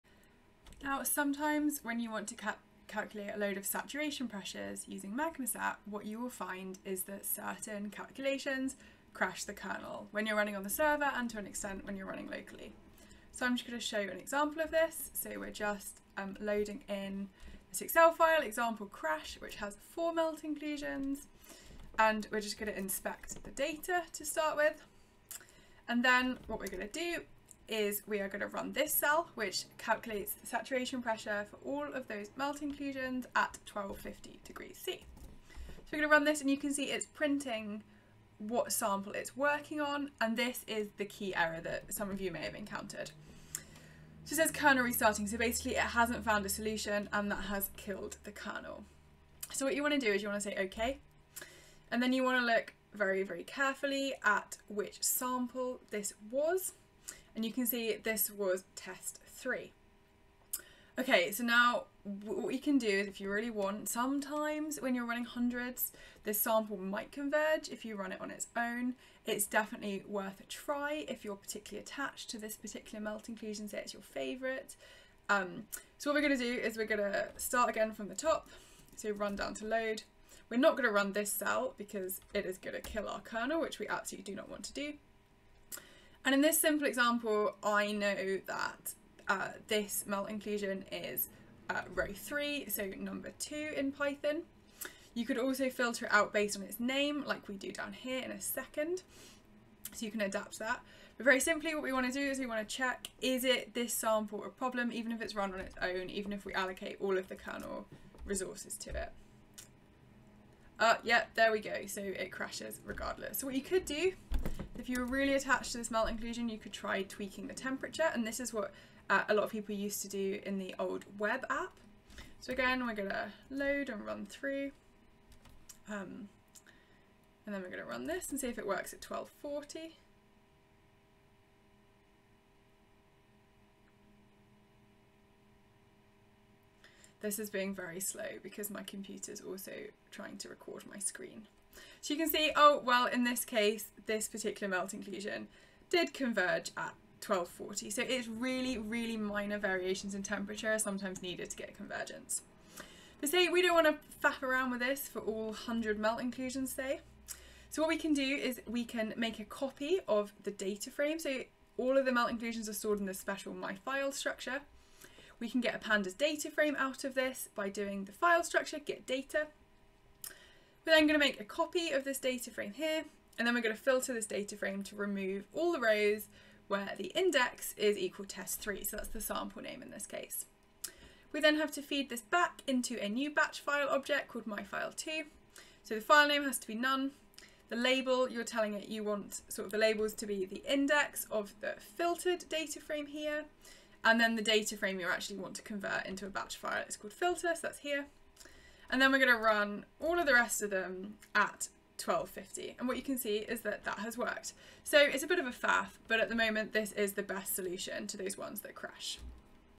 Solution to Magmasat "Kernel Restarting"
This video shows an error where the Kernel crashes for a MagmaSat calculation, and how users can filter their file to remove the melt inclusions which are causing the crash.